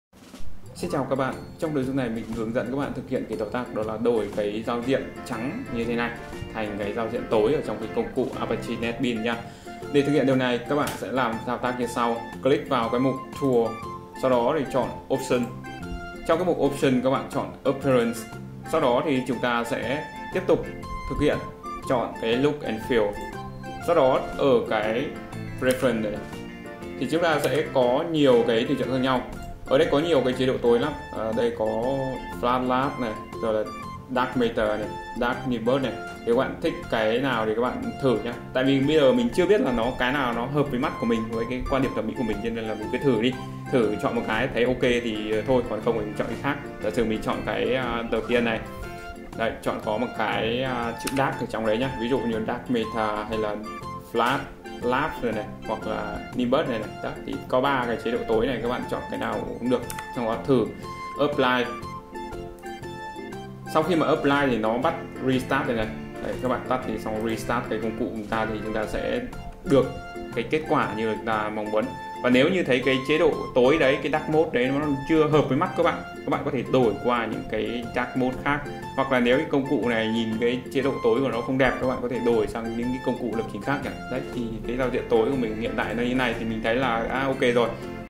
Đổi giao diện tối cho Apache Netbeans
Hướng dẫn đổi giao diện tối cho công cụ Apache Netbeans